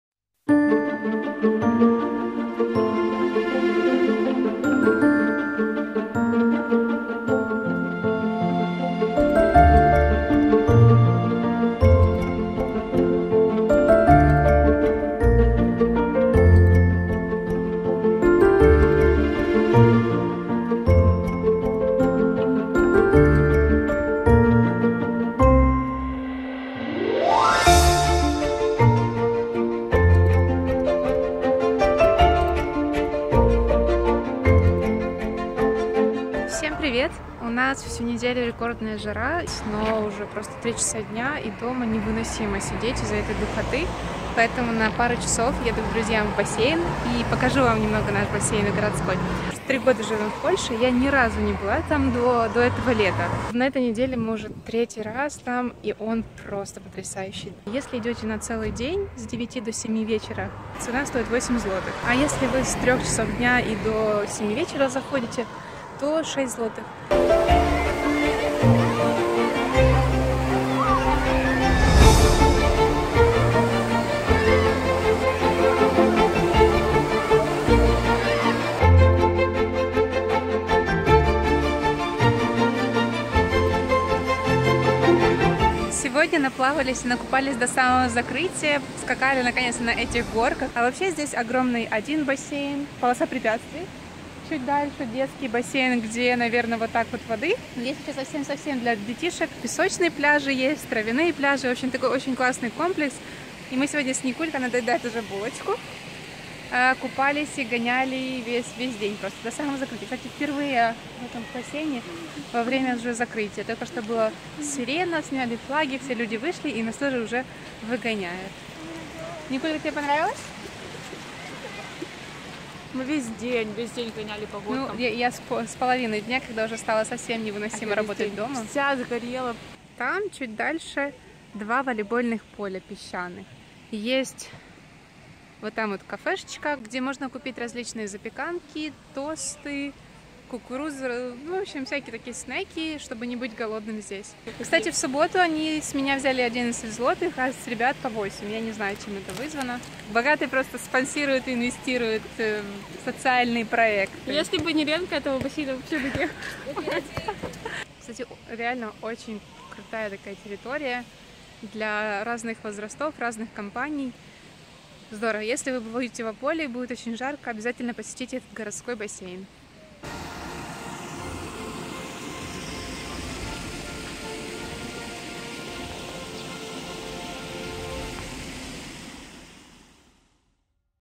Отдых | Один день в Польше
Вы на канале о жизни в Польше – Helen Mazanova w Polsce

В этом видео хочу показать вам городской бассейн под открытым небом в Польше. Прекрасный аттракцион для семейного отдыха или большой компании в Ополе.
Показываю один из трех городских бассейнов в Польше.  Цены, территория и возможности этого места постаралась  максимально показать в этом видео.

Proszę zobacz ten odcinek: "Narzekanie. Dlaczego nie narzekam"